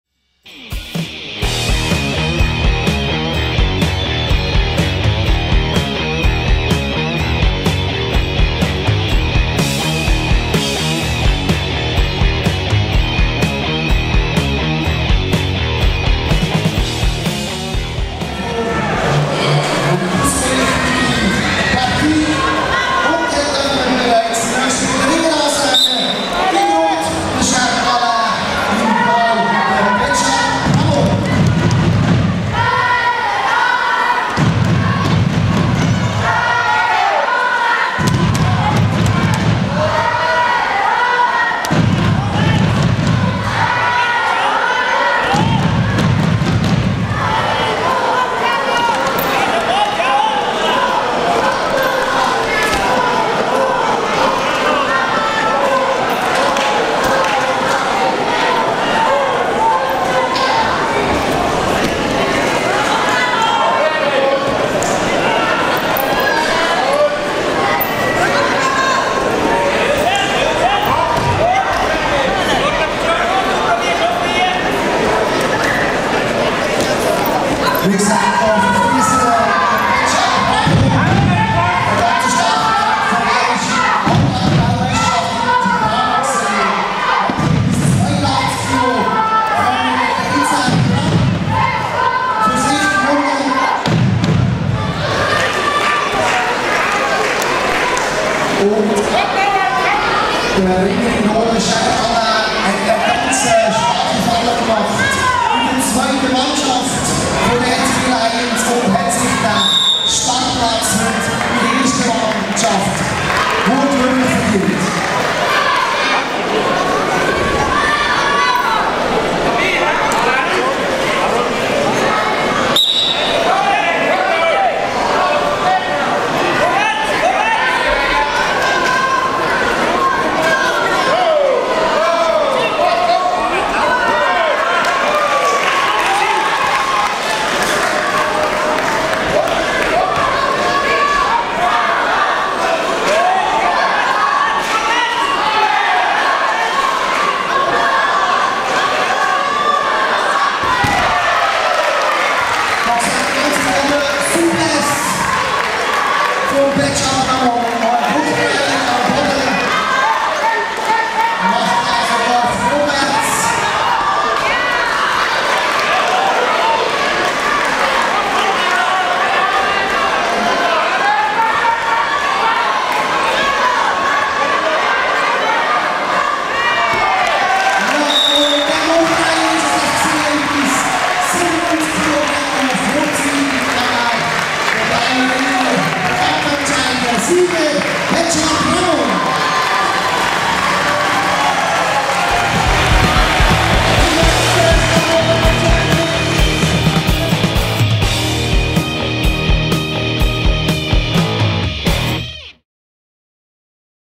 Schärer Alain (RCW) - Betschart Ramon (RSK) - Swiss Premium League
RC Willisau Lions - RS Kriessern
2. November 2024 20:00
BBZ Halle Willisau / LU
Kampfrichter: Kilian Motzer
650 Zuschauer